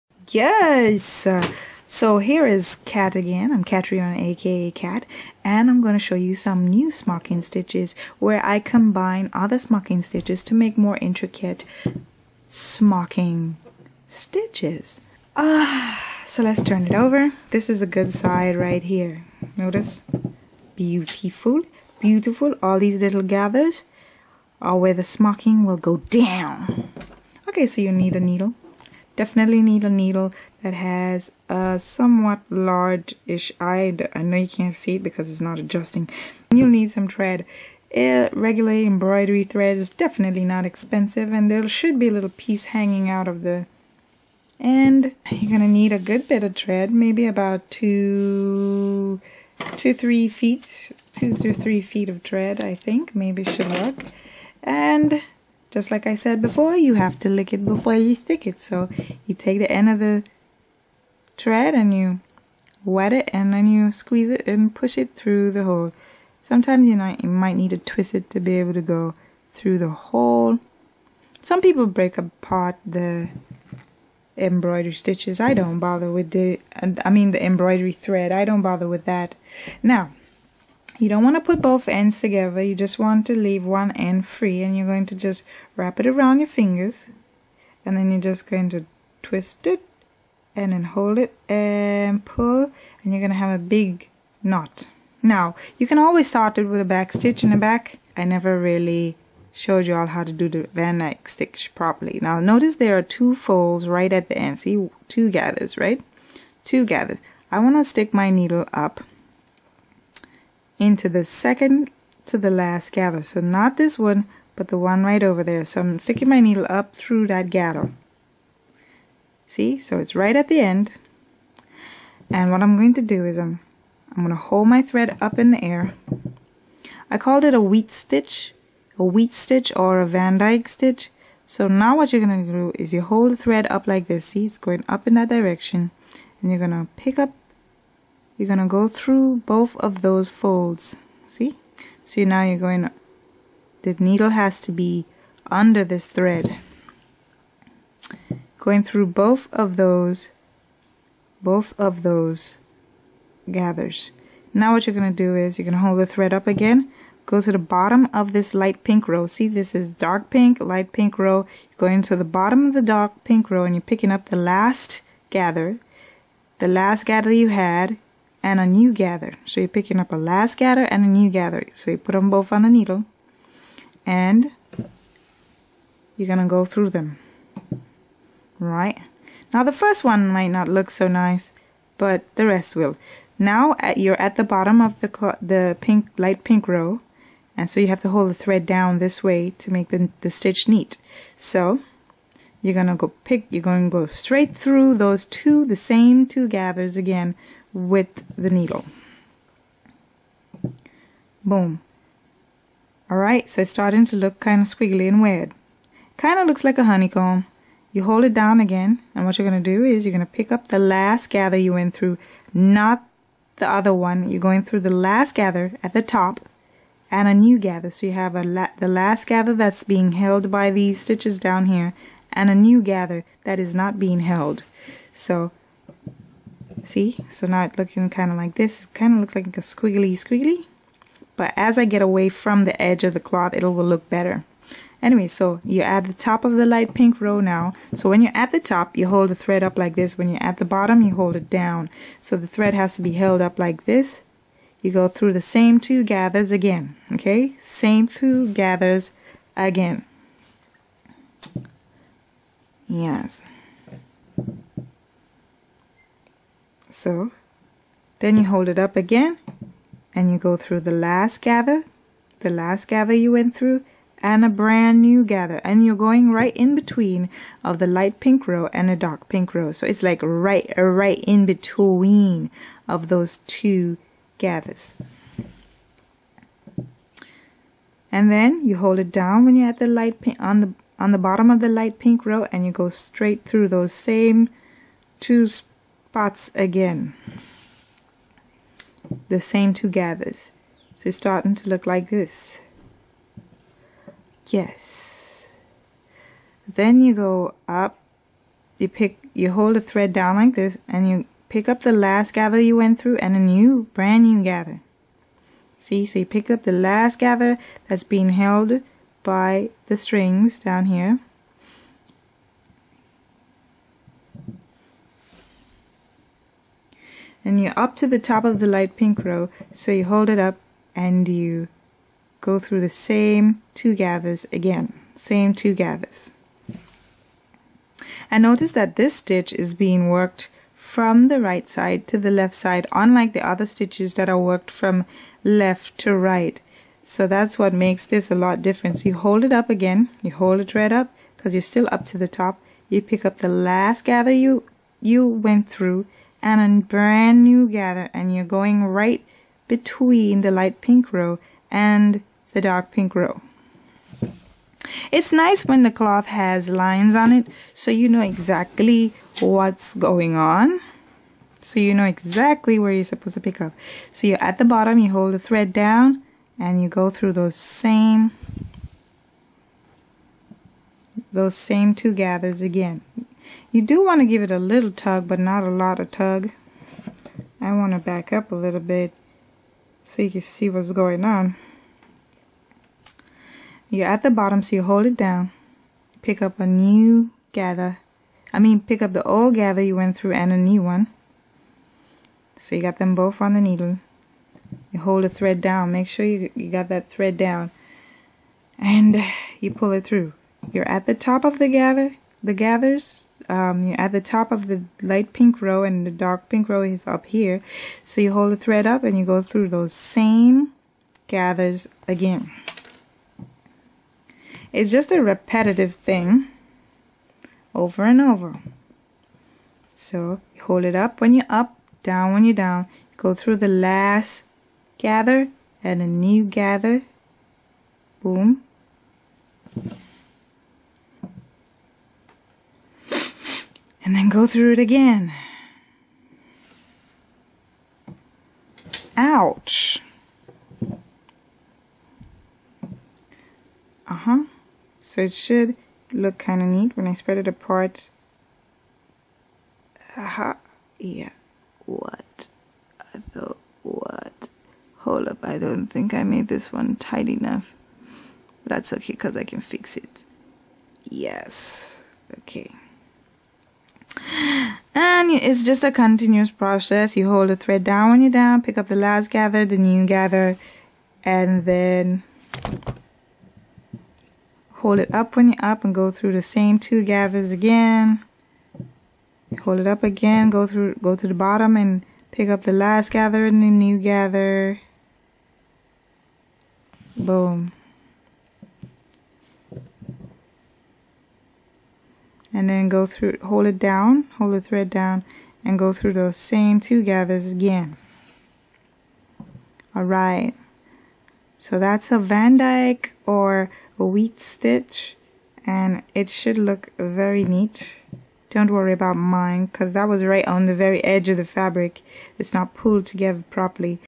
smocking stitch tutorial vandyke or wheat stitch
showing how to sew a Vandyke or wheat stitch